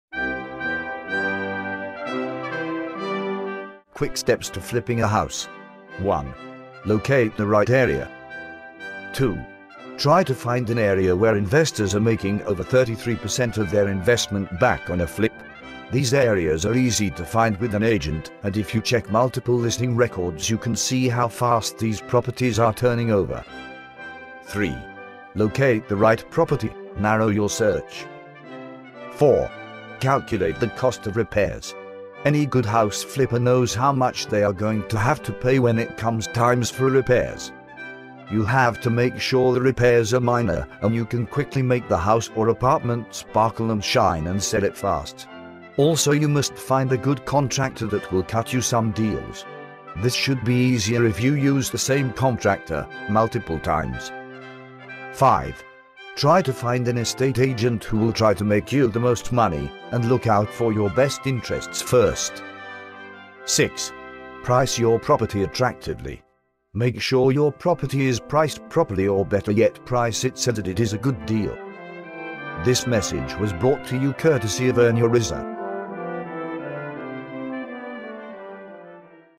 Joffrey Presents: Flipping A House
This video is a video blog on how to flip a house and make some profit.